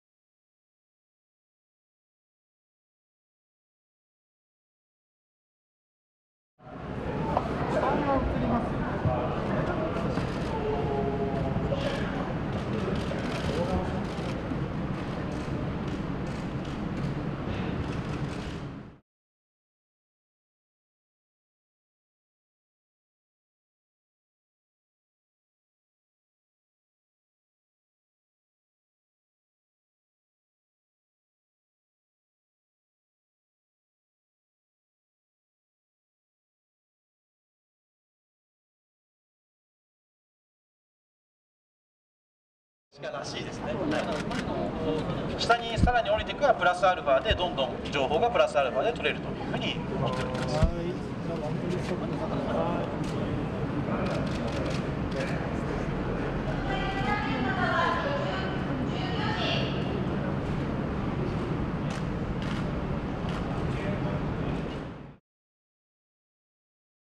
水中調査ロボ福島第１原発内部へ 東芝などが開発
東芝と技術研究組合国際廃炉研究開発機構（ＩＲＩＤ）は、冷却水がたまった福島第１原子力発電所３号機の原子炉格納容器内部を調査する小型ロボットを開発した。燃料デブリを取り出すための計画づくりに役立てる。

【日経電子版映像ページはこちら】 ※日本経済新聞社の動画について改ざんや、許可なく商用・営利 目的で利用することを禁じます。